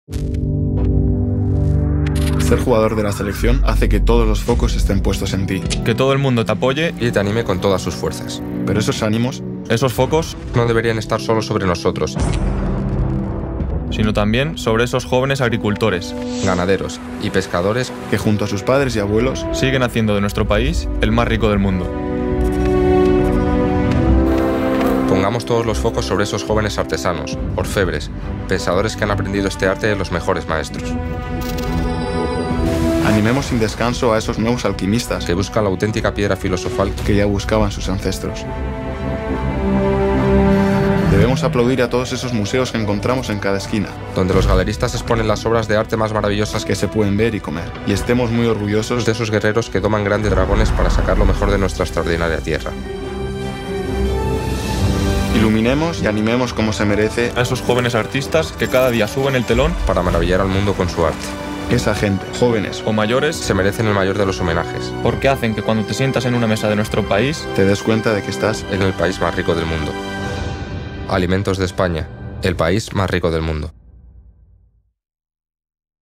CAMPAÑA ALIMENTOS DE ESPAÑA - LA SELECCIÓN
Desde el Gourmet de la Roja nos sentimos orgullosos de participar con la RFEF en este proyecto de marca España.